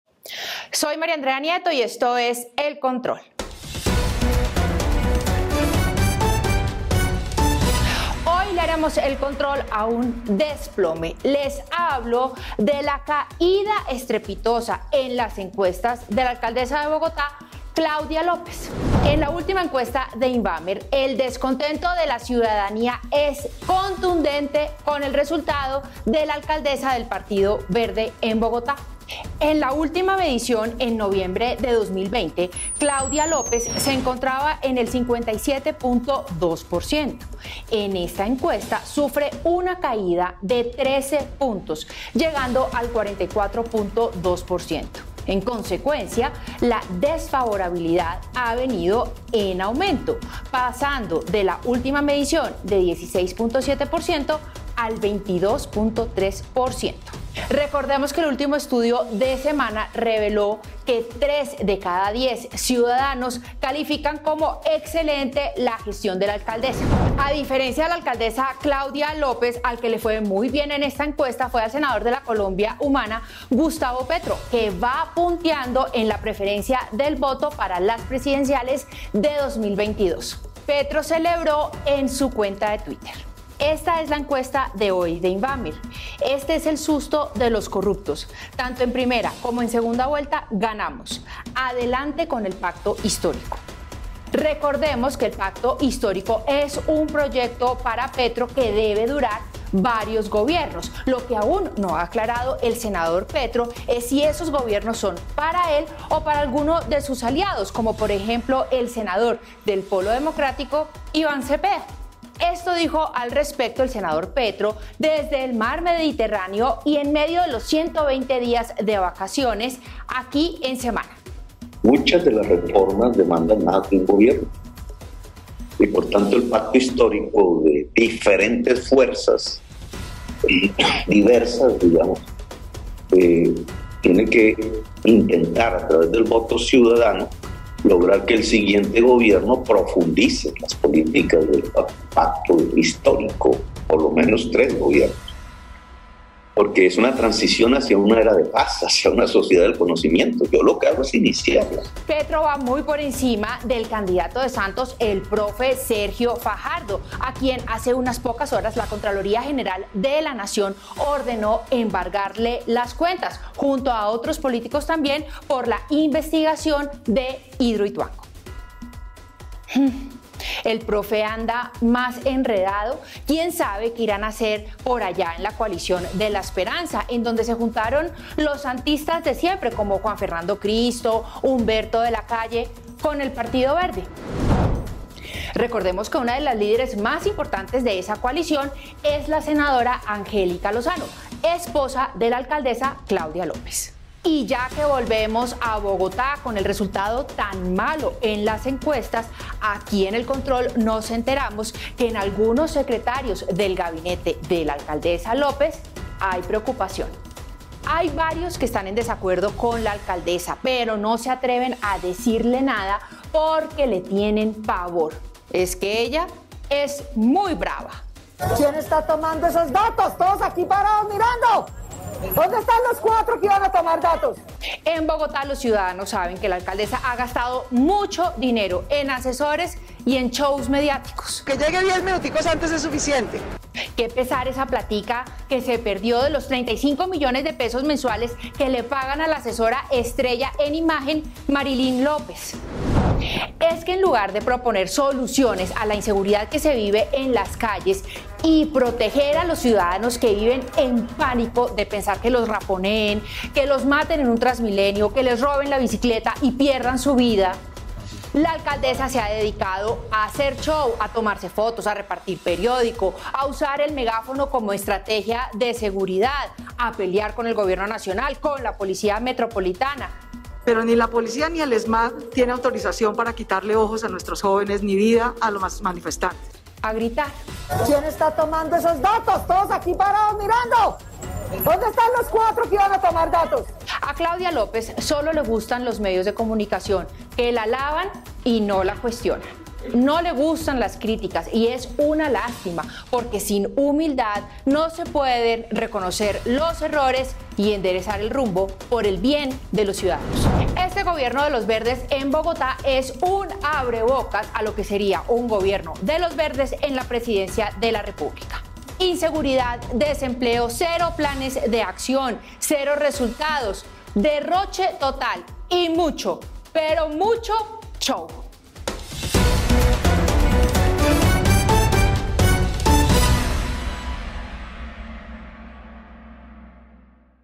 El Control al “desplome” de Claudia López en las encuestas
“Inseguridad, desempleo, cero planes de acción, cero resultados, derroche a la lata y mucho, pero mucho show": María Andrea Nieto le hace ElControl al “desplome” de Claudia López en las encuestas.